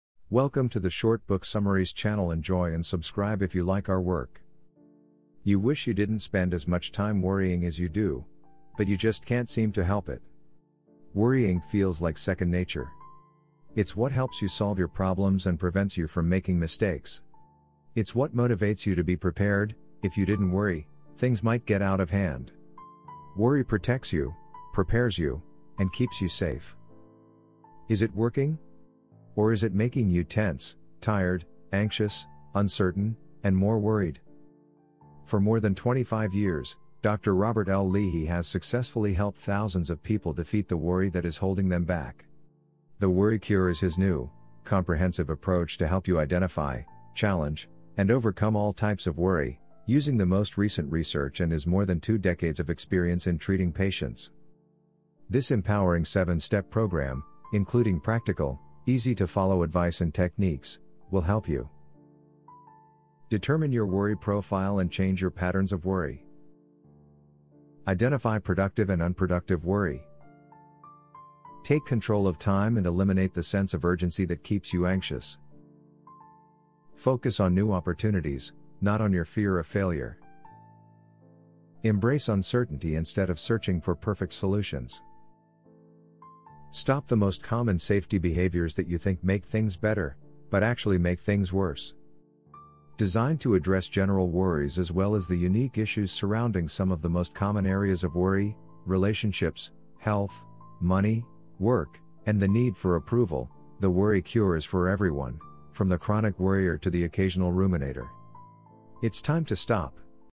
Short Book Summary of The Worry Cure Seven Steps to Stop Worry from Stopping You by Robert L Leahy
You wish you didn’t spend as much time worrying as you do, but you just can’t seem to help it. Worrying feels like second nature. It’s what helps you solve your problems and prevents you from making mistakes. It’s what motivates you to be prepared—if you didn’t worry, things might get out of hand. Worry protects you, prepares you, and keeps you safe.

Is it working? Or is it making you tense, tired, anxious, uncertain—and more worried?

For more than twenty-five years, Dr. Robert L. Leahy has successfully helped thousands of people defeat the worry that is holding them back. The Worry Cure is his new, comprehensive approach to help you identify, challenge, and overcome all types of worry, using the most recent research and his more than two decades of experience in treating patients.

This empowering seven-step program, including practical, easy-to-follow advice and techniques, will help you:

• Determine your “worry profile” and change your patterns of worry

• Identify productive and unproductive worry

• Take control of time and eliminate the sense of urgency that keeps you anxious

• Focus on new opportunities—not on your fear of failure

• Embrace uncertainty instead of searching for perfect solutions

• Stop the most common safety behaviors that you think make things better—but actually make things worse

Designed to address general worries as well as the unique issues surrounding some of the most common areas of worry—relationships, health, money, work, and the need for approval—The Worry Cure is for everyone, from the chronic worrier to the occasional ruminator. It’s time to stop